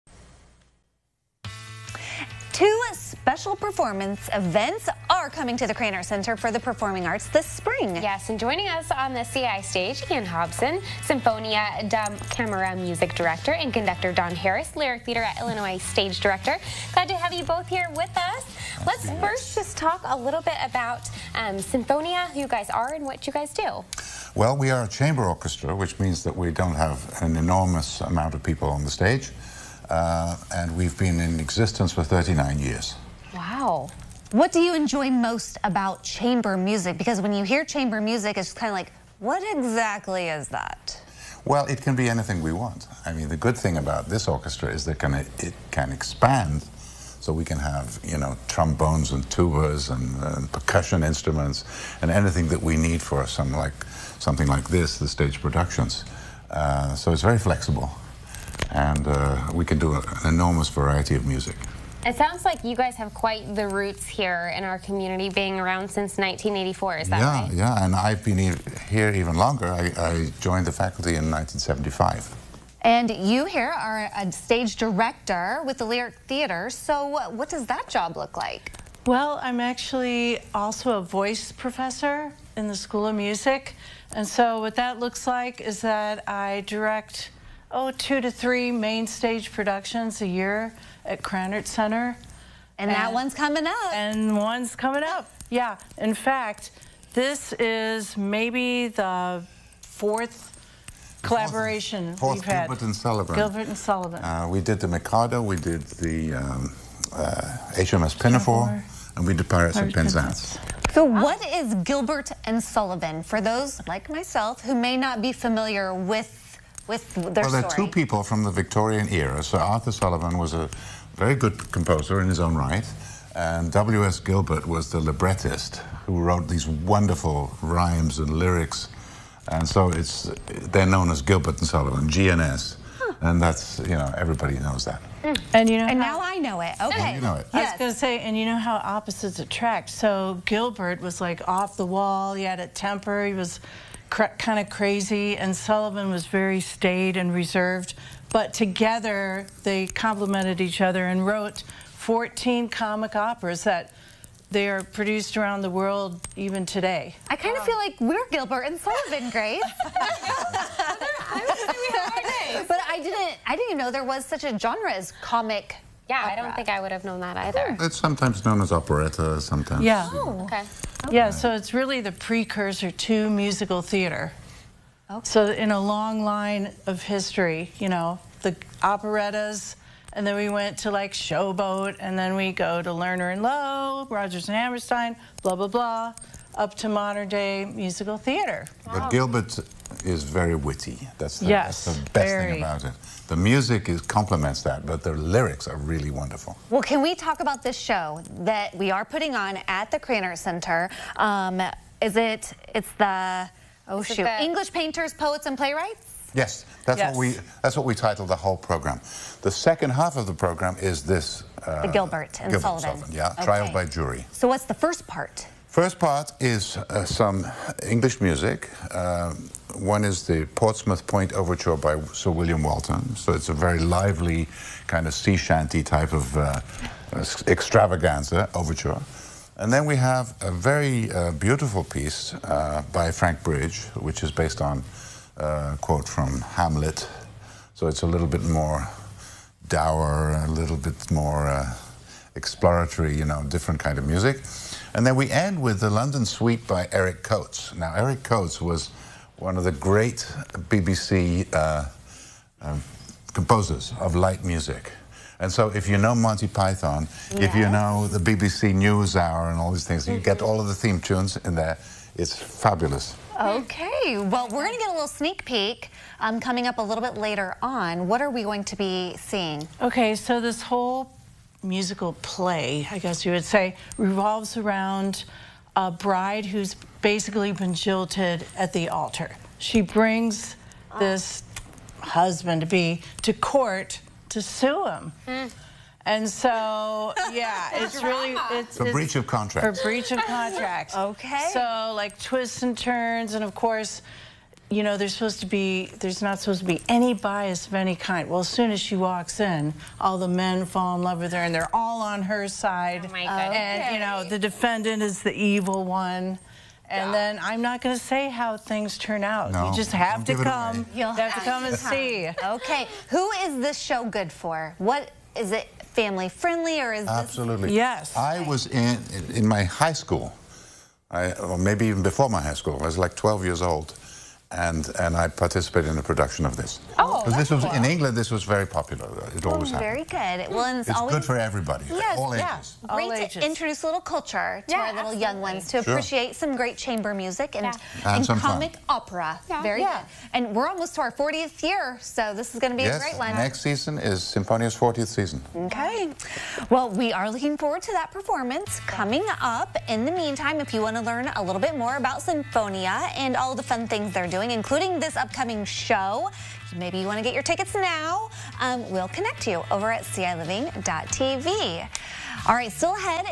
ciLiving Sinfonia da Camera & Lyric Theatre at Illinois Theatre
ciLiving previews a special show coming to The Krannert Center for the Performing Arts